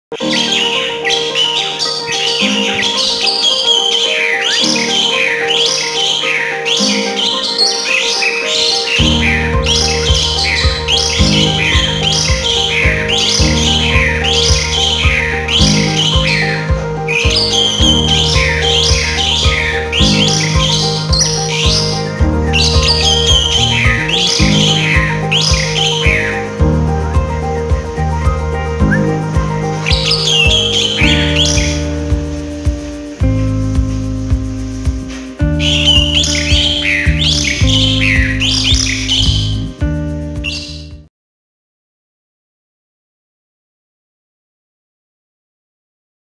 Suara Kicauan Burung Jalak Suren Gacor, untuk Masteran Suara Burung !!!
Memiliki burung Jalak Suren yang gacor memang menyenangkan. Selain suaranya dapat dinikmati juga menjadi tanda bahwa burung tsb.siap breeding.

Burung harus distimulasi dengan suara burung lain. Baik lewat MP3 atau HP maupun burung asli yang dijadikan master.